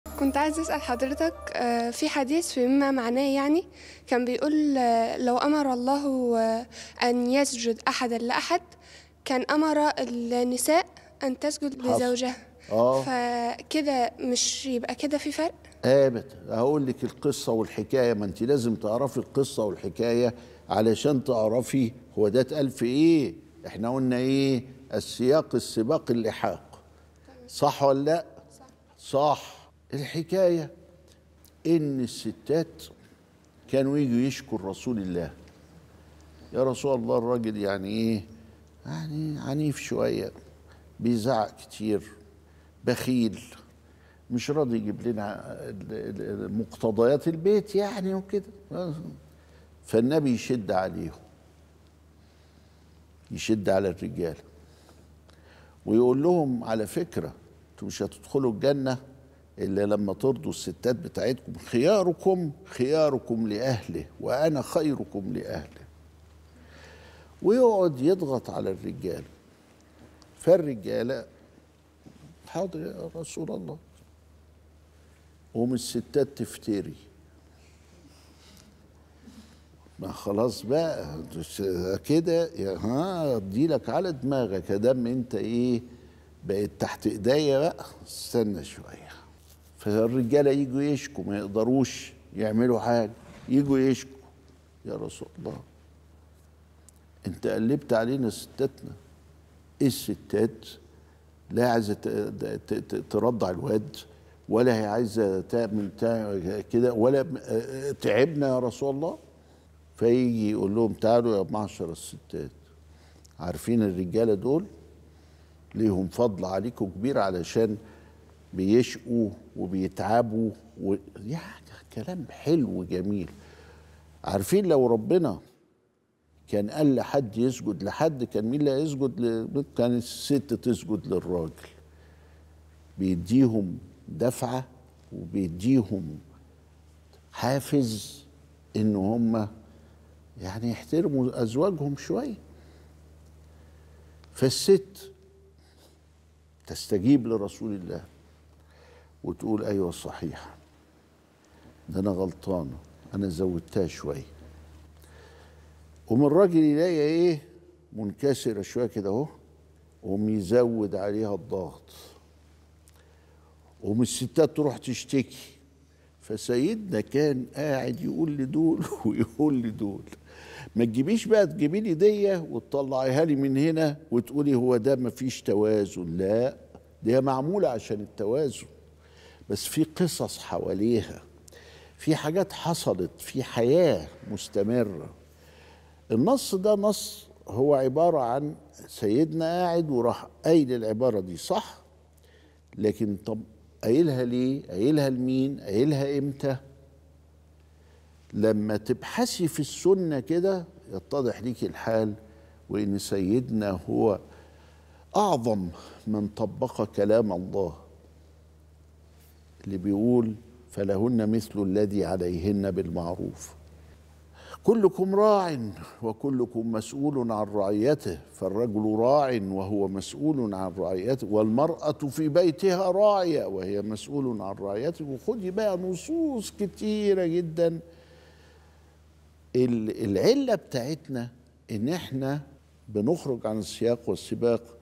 ما الرد علي التفرقة بين الرجل والمرأة في حديث "سجود المرأة لزوجها"؟ .. د. علي جمعة: ارجعوا للسياق
نور الدين | ما الرد علي التفرقة بين الرجل والمرأة في حديث "سجود المرأة لزوجها"؟ .. د. علي جمعة: ارجعوا للسياق واللحاق